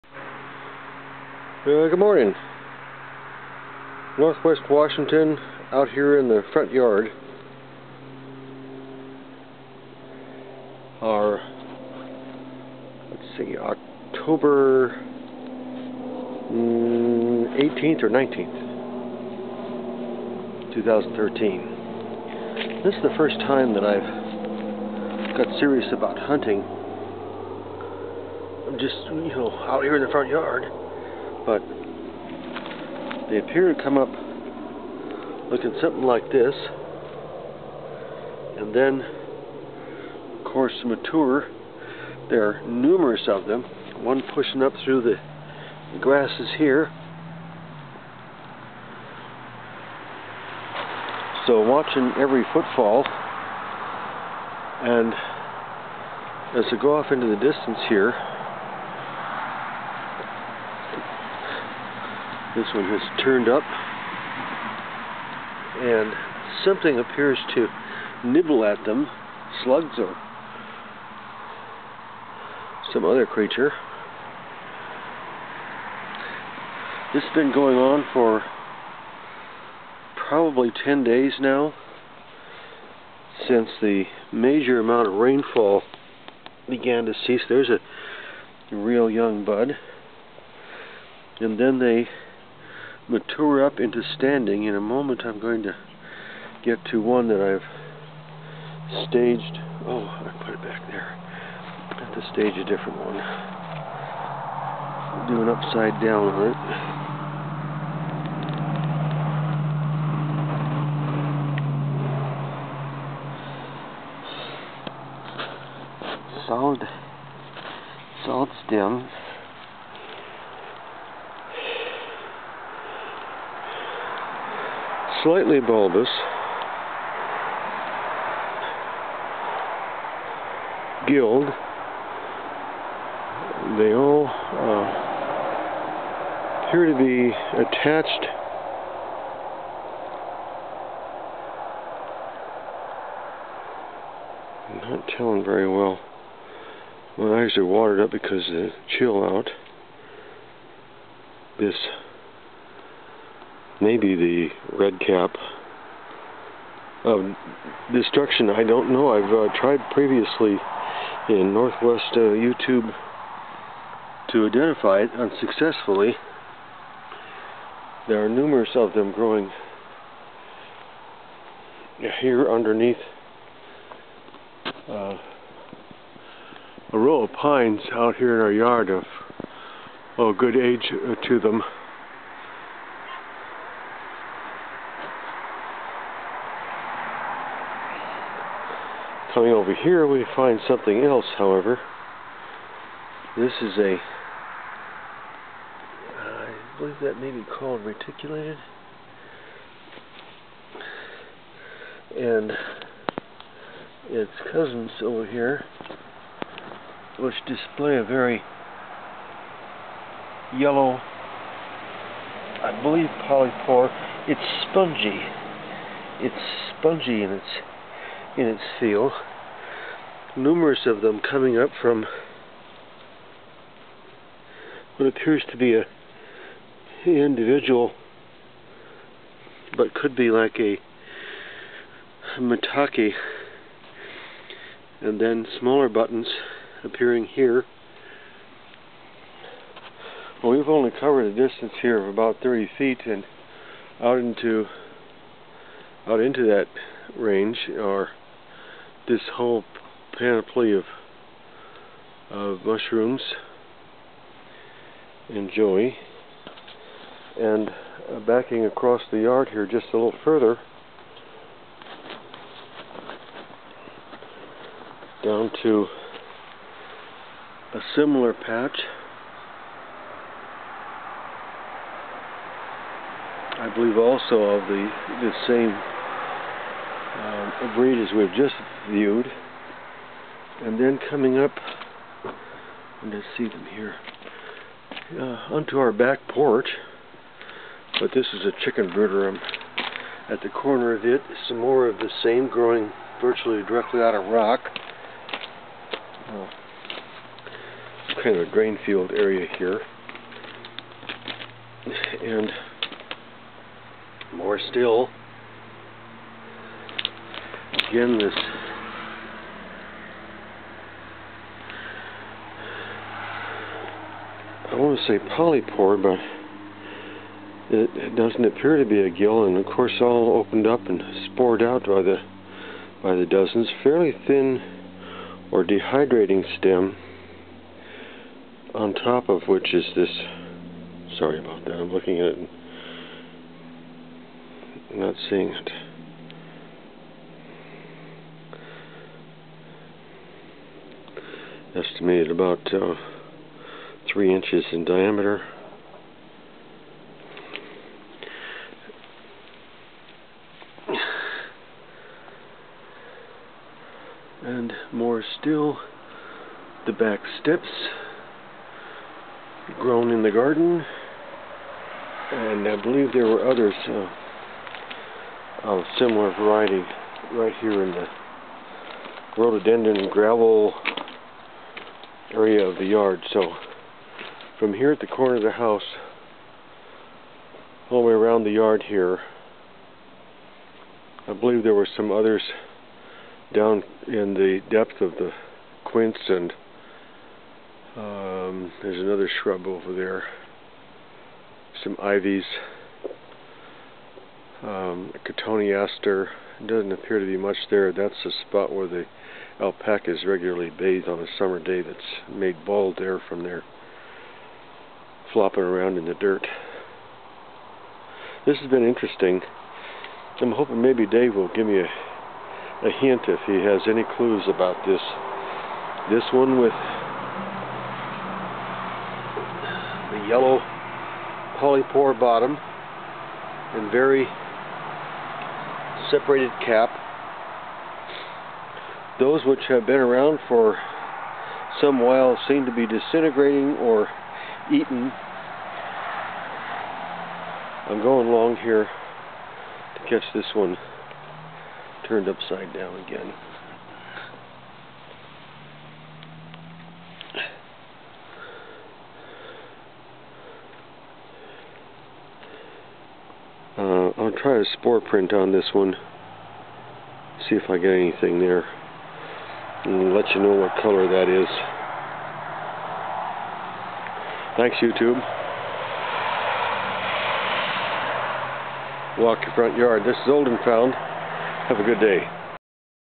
Mushrooms PNW in the Yard #1 10-19-13
The two of us have been witness this year to a populating of mushrooms out the windows of the kitchen and living room. I will continue to research what may be 3 or more species, thus far without success. I am way amateur novice fledgling nearing 70 with 6 months mushrooming experience. We live in the Western foot hills of the Cascade Mountains. Heavy rains for a week abated about 10 days ago. Since then we are experiencing foggy nights and mornings or full days, with intermittent full sun mid days. I am so busy with farm projects, I need to take   a walk through the 3 1/2 acre pasture and woods out back.